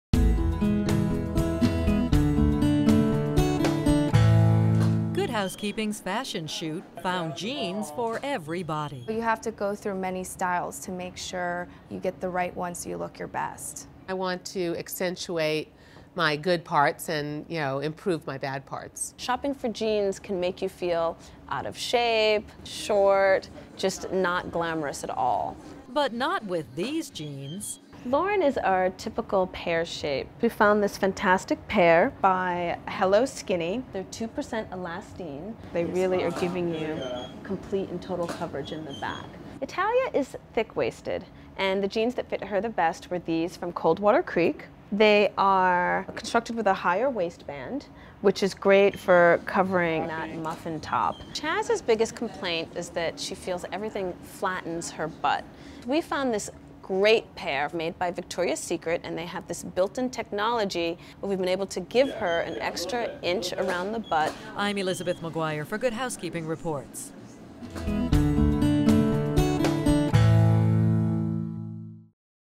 Best Jeans For Your Body
Whether you're petite, pear-shaped, or plus-size, we've got the right jeans for your body.